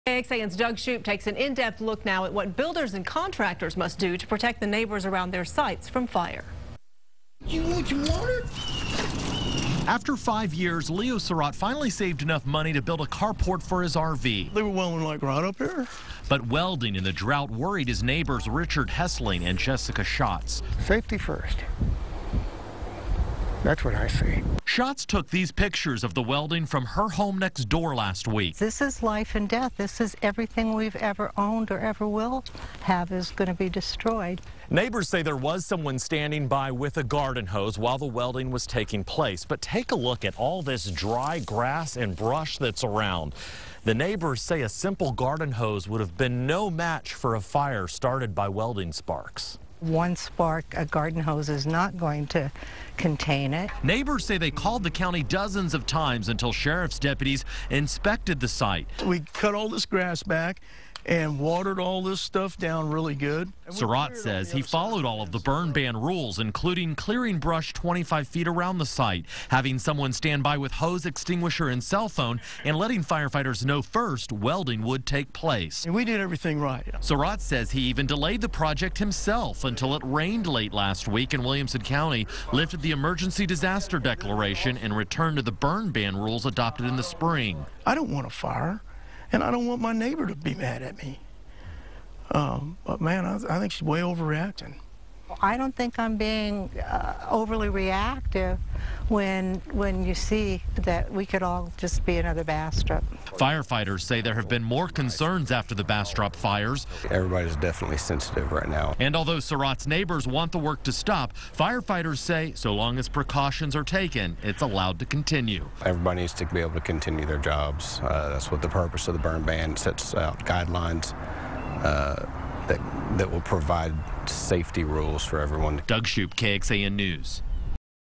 Neighbors worry about welding in Williamson County
County firefighters say as long as people follow burn ban rules, work can continue.